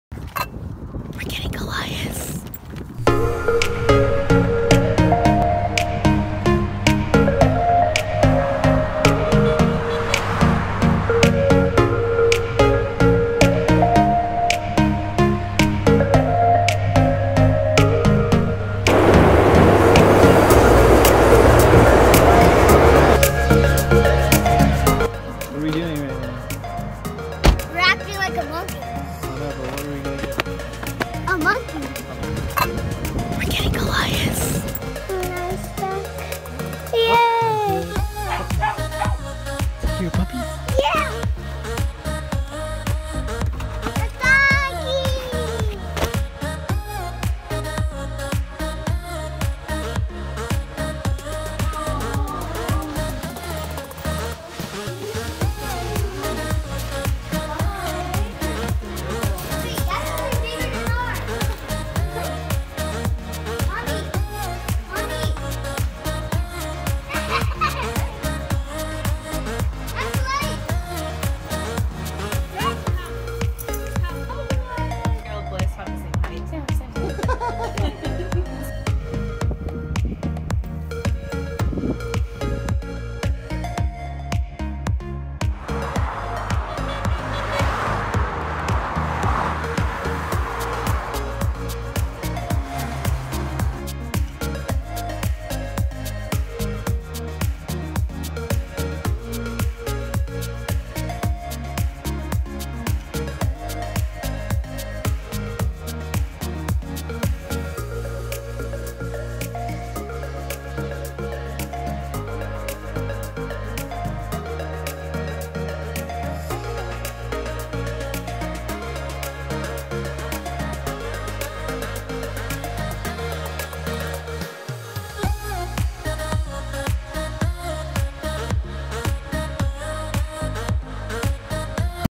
Trip from San Diego to Salt Lake City to get a Weimaraner
We took a road trip from San Diego to Salt Lake City to get our little puppy. We drove through snow storms and even got pulled over by the cops, but we made it! Got the puppy.

All video from iPhone 7.